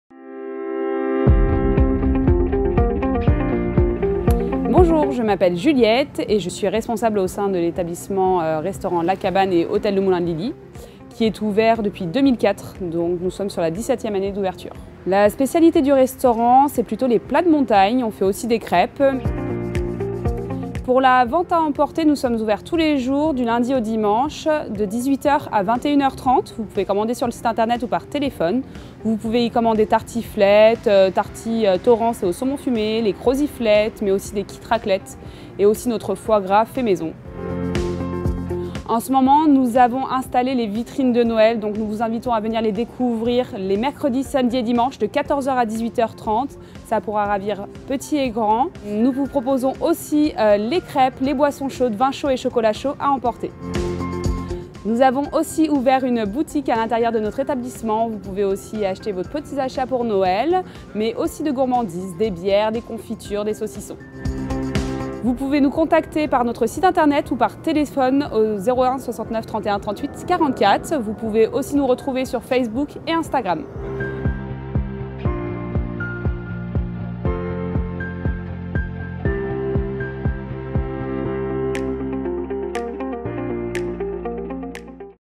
Restaurant La Cabane
Comme de nombreux restaurateurs palaisiens, La Cabane s'adapte au contexte sanitaire pour vous continuer de vous servir et vous aider à préparer les fêtes ! L'établissement vous propose une restauration à emporter, et a mis en place une boutique à l'intérieur du restaurant. Retrouvez sur notre site la liste des commerçants utilisant le système Click and Collect